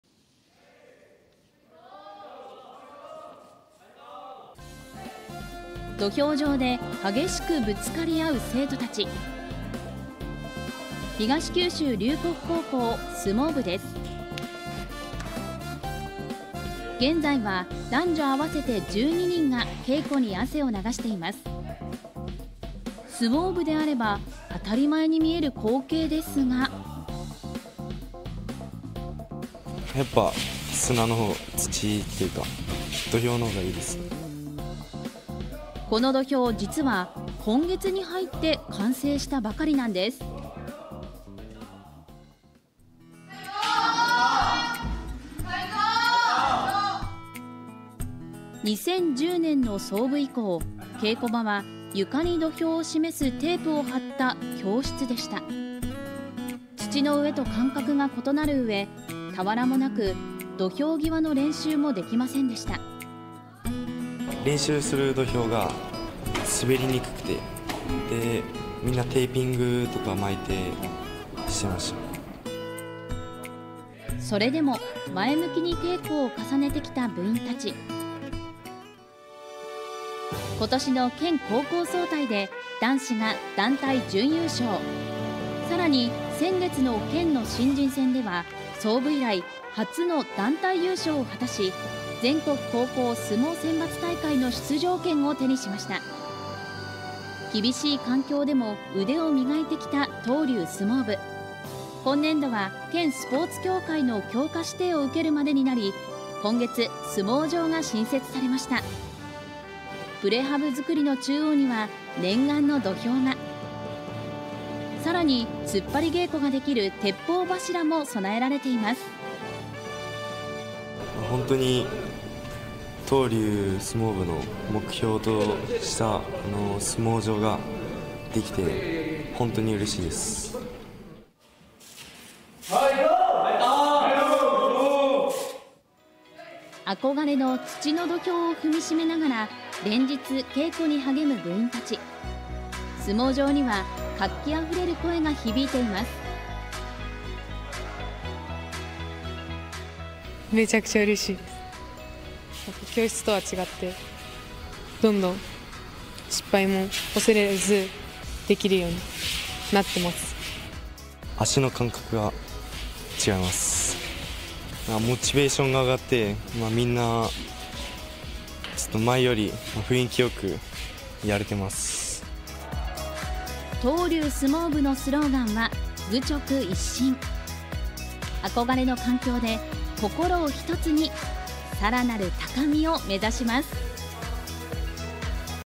「教室」で稽古の相撲部に待望の土俵「愚直一心」で稽古に励む東龍相撲部【大分】 (22/11/30 20:00)
憧れの土俵

大分県中津市の東九州龍谷高校に１１月、相撲場が完成しました。
これまでは教室で稽古に励んでいた部員たち、憧れの土俵を踏みしめながら高みを目指しています。

当たり前の光景　実は

土俵上で激しくぶつかり合う生徒たち。
東九州龍谷高校、相撲部です。

現在は男女合わせて１２人が稽古に汗を流しています。
相撲部であれば当たり前に見える光景ですが…

土俵が完成

ーー生徒
「やっぱり砂の土というか土俵の方が良い」

この土俵、実は１１月に入って完成したばかりなんです。

これまでの稽古場は教室

２０１０年の創部以降、稽古場は床に土俵を示すテープを貼った教室でした。
土の上と感覚が異なる上、俵も無く土俵際の練習もできませんでした。

前向きな稽古を重ね相撲場を新設

ーー東九州龍谷高校　相撲部　右田英五郎主将（２年）
「練習する土俵がコンクリートで滑りにくくて皆テーピングを巻いてしていた」

それでも前向きに稽古を重ねてきた部員たち。
ことしの県高校総体で男子が団体準優勝。さらに１０月の県の新人戦では創部以来、初の団体優勝を果たし、全国高校相撲選抜大会の出場権を手にしました。

厳しい環境でも腕を磨いてきた東龍相撲部。
今年度は県スポーツ協会の強化指定を受けるまでになり１１月、相撲場が新設されました。

待望の土俵

プレハブ造りの中央には念願の土俵が。
さらに突っ張り稽古ができる「鉄砲柱」も備えられています。

ーー部員
「本当に東龍相撲部の目標とした相撲場ができて本当にうれしい」

憧れの土の土俵を踏みしめながら連日、稽古に励む部員たち。
相撲場には活気あふれる声が響いています。

ーー部員
「めちゃくちゃうれしい。教室とは違ってどんどん失敗を恐れずできるようになっている」

ーー右田英五郎主将
「足の感覚が違う。モチベーションが上がってみんな前より雰囲気良くやれている」

東龍相撲部のスローガンは「愚直一心」。
憧れの環境で心を一つに更なる高みを目指します。